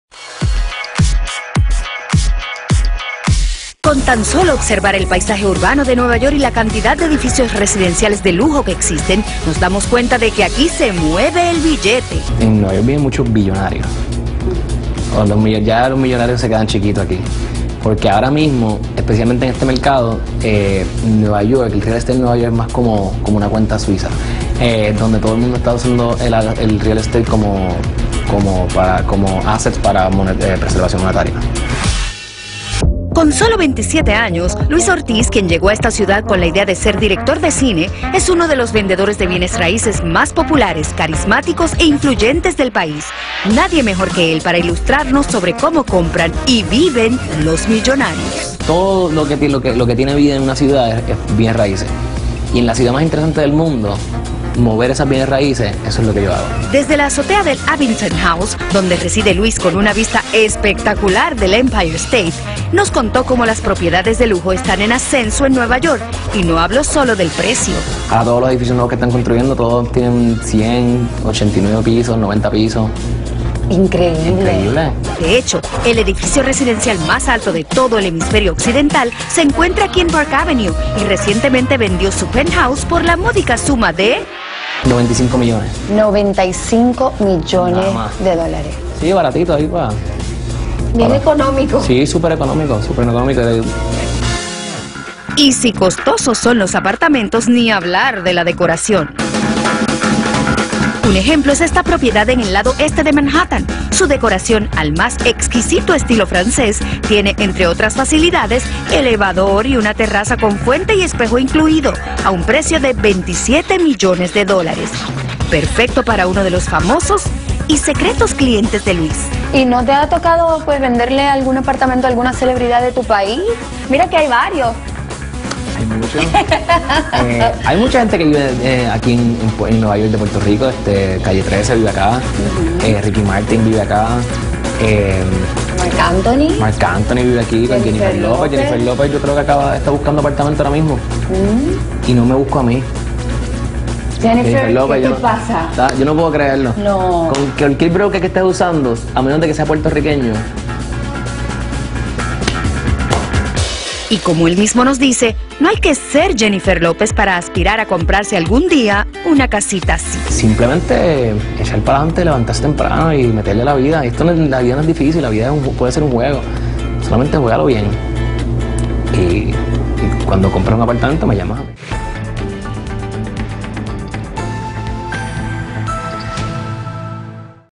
El vendedor de bienes raíces boricua más cotizado de NY
Luis Ortíz quería ser director de cine, pero por cosas del destino se convirtió en el vendedor de bienes raíces más cotizado en Nueva York.